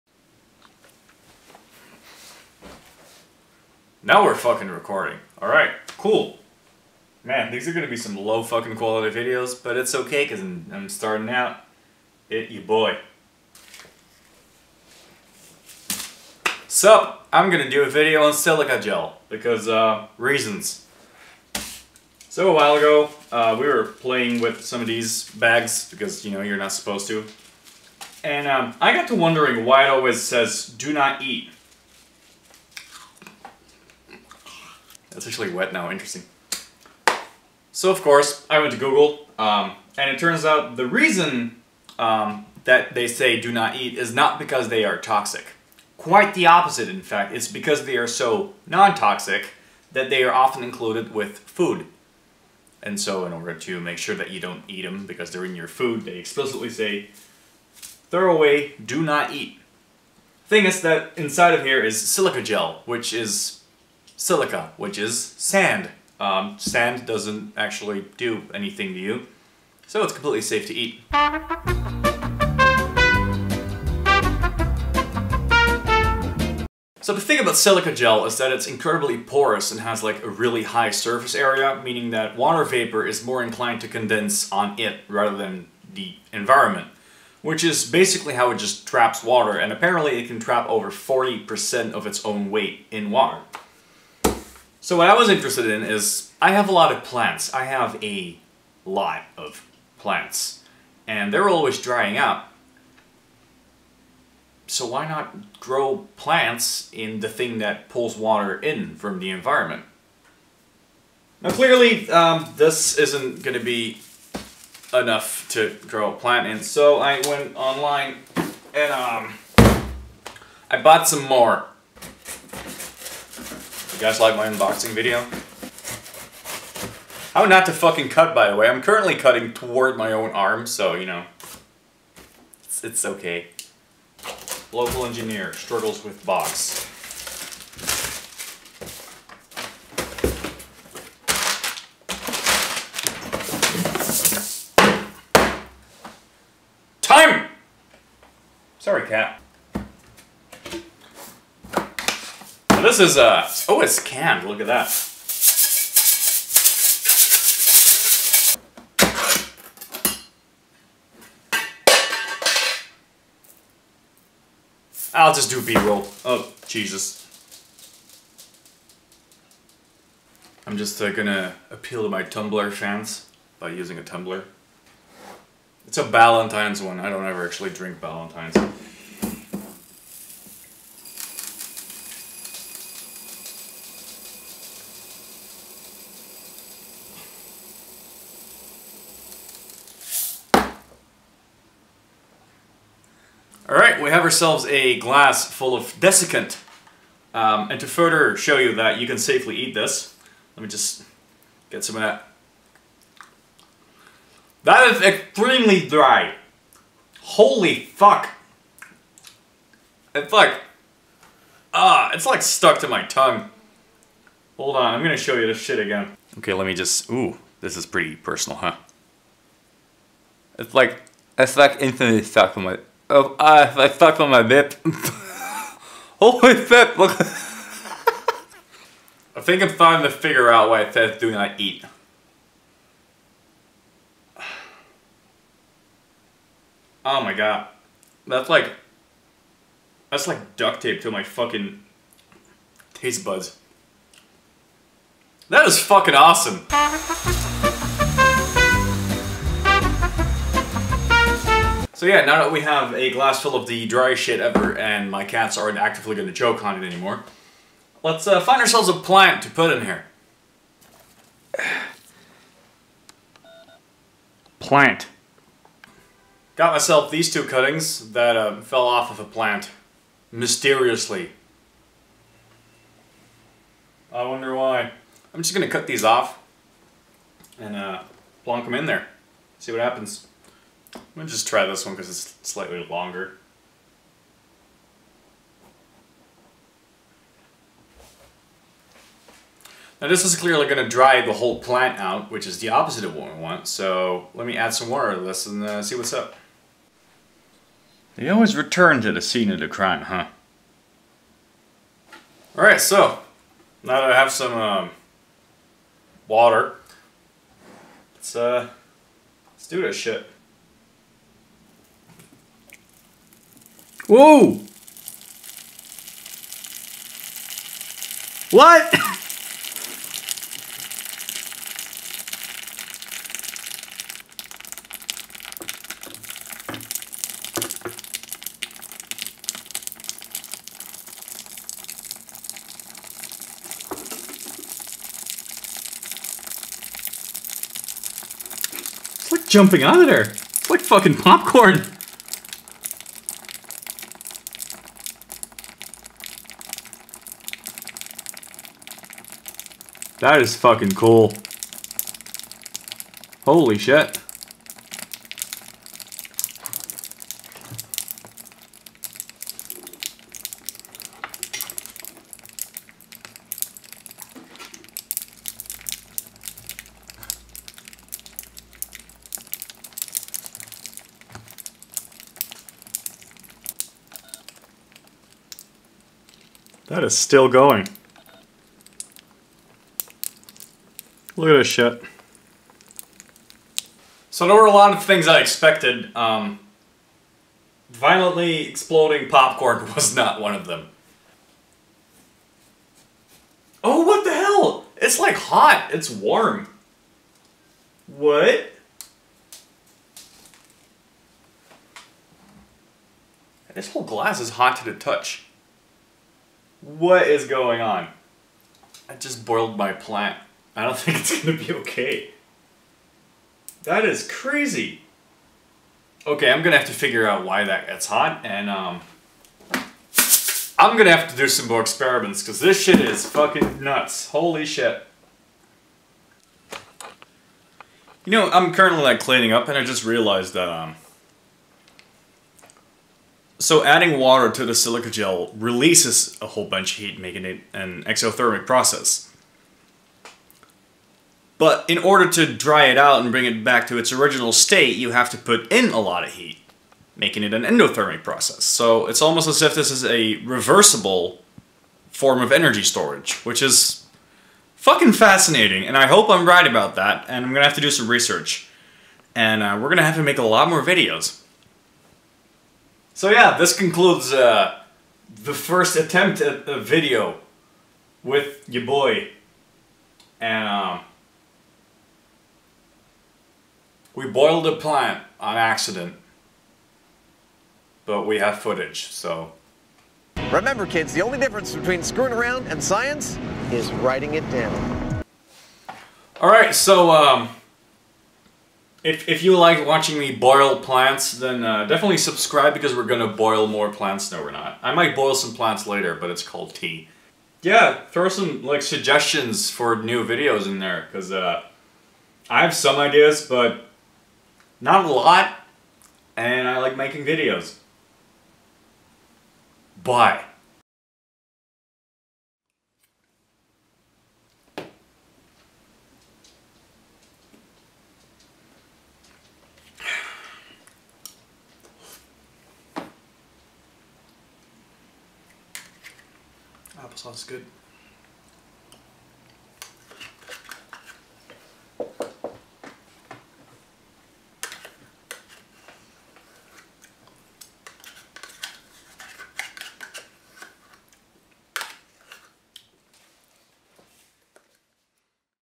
Growing plants in silica gel, and how to accidentally boil them in the process
For a while now I've been thinking about the possibility of growing plants in silica gel desiccant given its high moisture retaining capability (something I was hoping the root systems can overcome through capillary action). However, in doing so I discovered the surprisingly exothermic properties of silica gel.